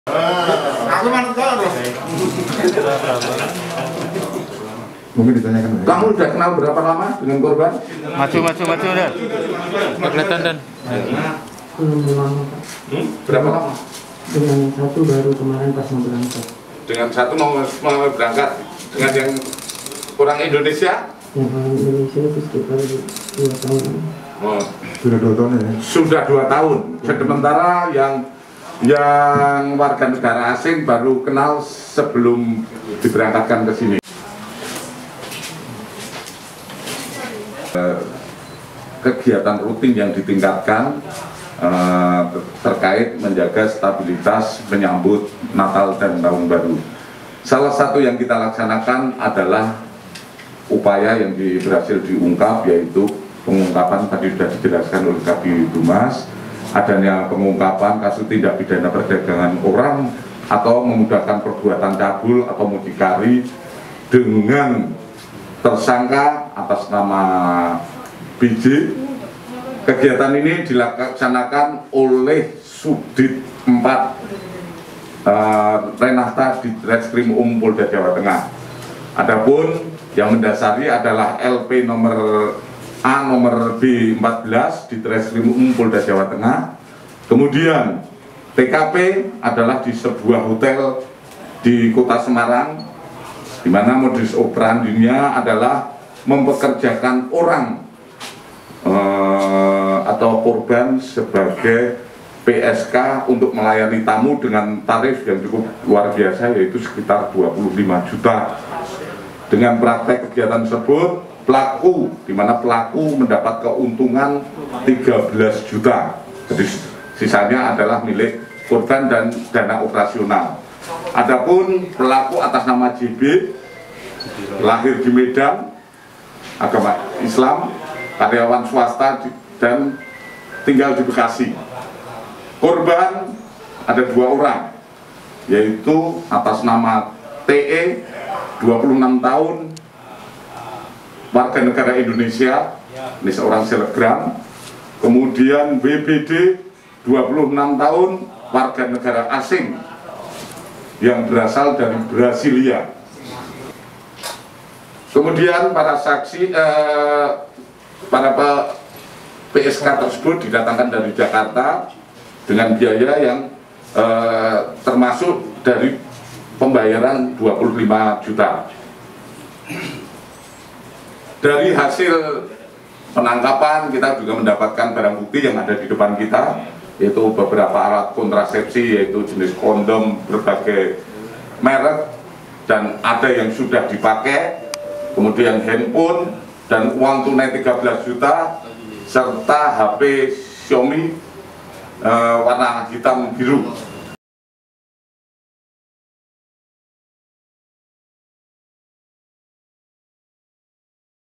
Selebgram TE Ditangkap di Hotel Semarang, Diduga Terlibat Prostitusi
RADAR SEMARANG TV

PT. SEMARANG INTERMEDIA DIGITAL
Jl. Veteran no. 55, RT005/RW006, Lempongsari, Gajahmungkur, Semarang, 50231